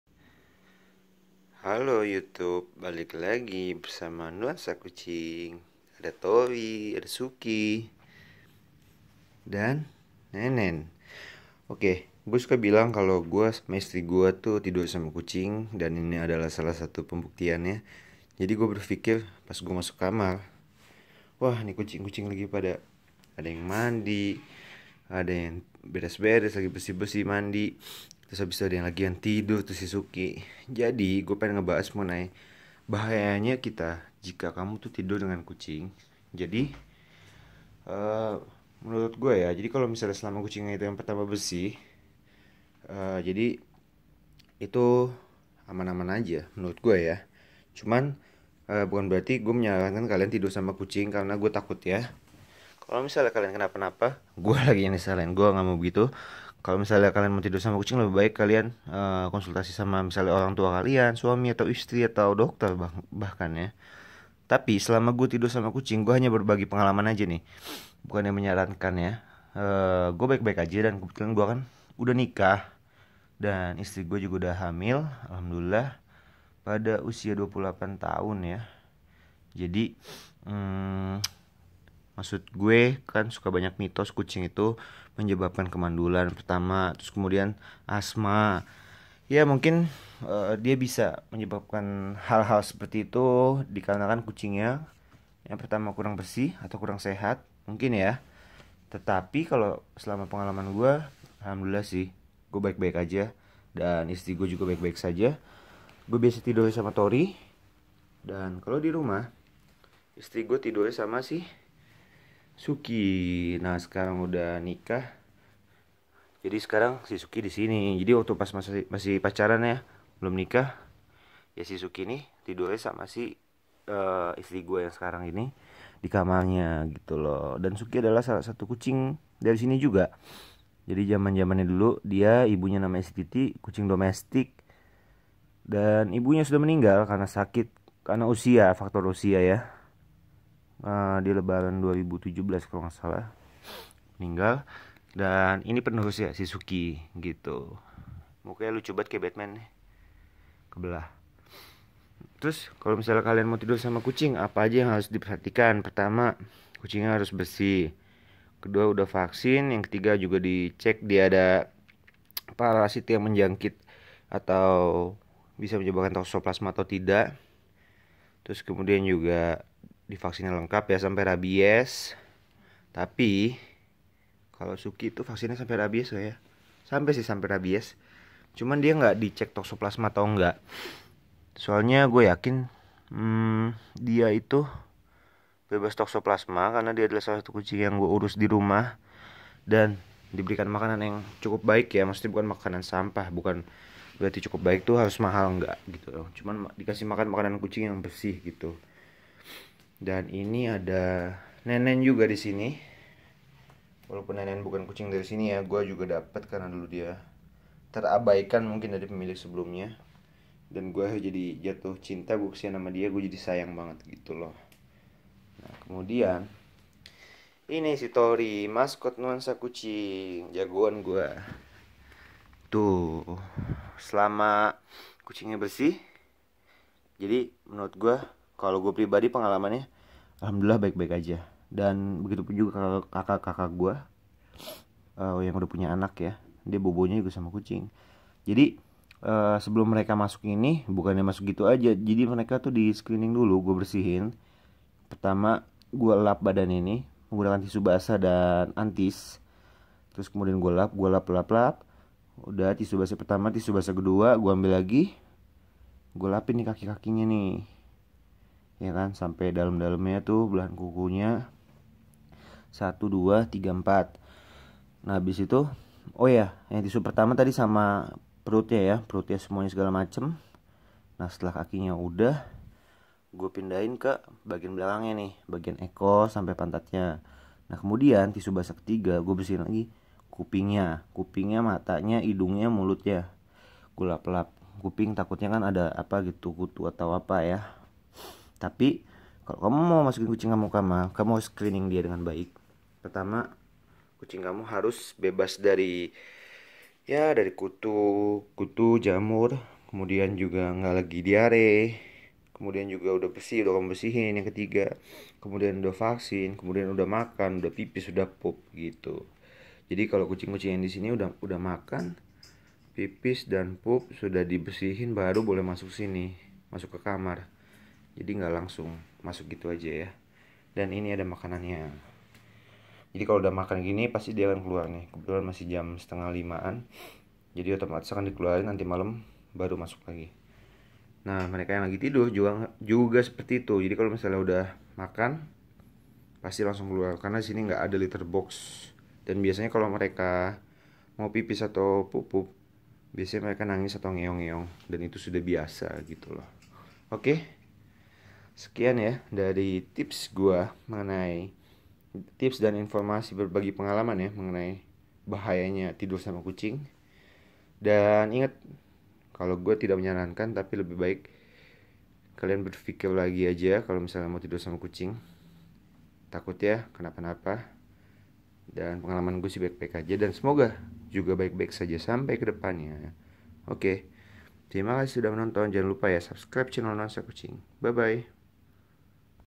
Bahaya Tidur Dengan Kucing?
Endorse, Business & Service Contact / whatssapp : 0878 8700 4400

Bahaya. Jika kucing tersebut kotor dan tidak sehat, oleh karena itu memperhatikan kebersihan dan kesehatan kucing kamu perlu loh.